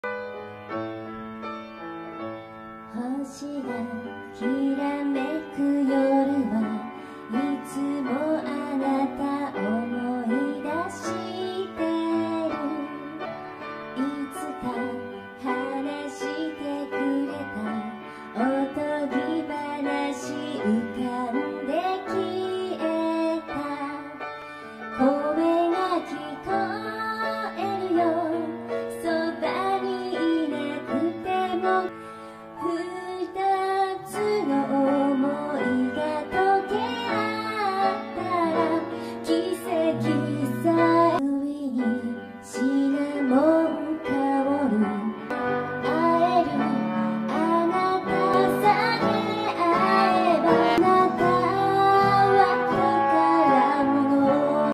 宍戸留美にとっての大切なアニメ作品の歌
並木のり子さんのアニメデビュー記念周年イベントでcover曲を歌わせて頂きました。